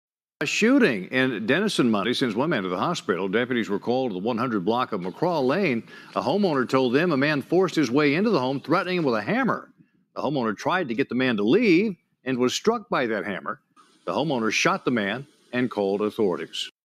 Homeowner Shoots Hammer-Wielding Intruder In Grayson County
A hammer-wielding man was shot and critically wounded by a Denison-area homeowner on Monday.